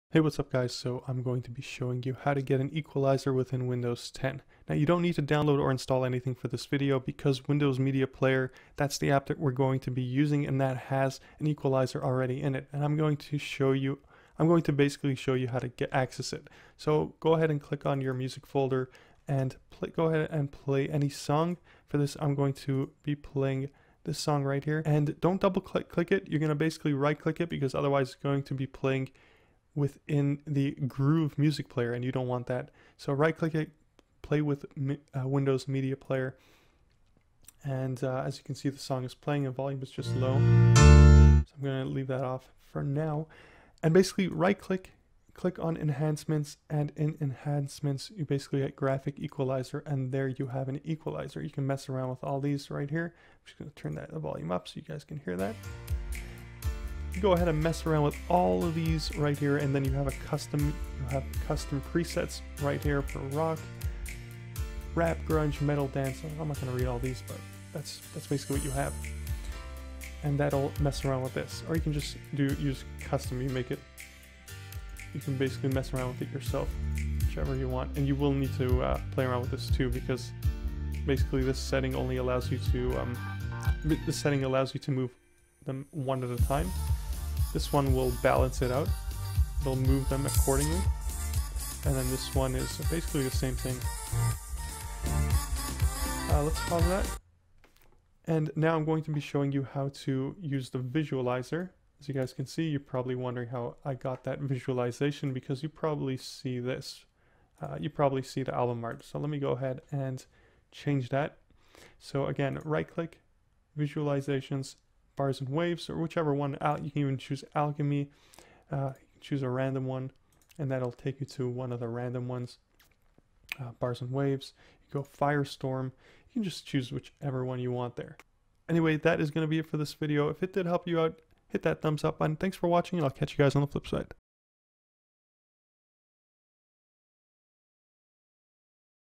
Music Equalizer Windows 10
If you are you getting the big window  with album and all the lists on the side - then on the bottom right corner you'll see a small icon (an arrow with 3 boxes) that if you hover over it will say "switch to now playing" click on it! that should switch it to the smaller "Now playing" screen mode.

How to add Music Equalizer in Windows 10
Windows Media Player has a built in Music equalizer. and in this video I'm going to show you guys how to access it.

The long forgotten Windows Media Player Equalizer :)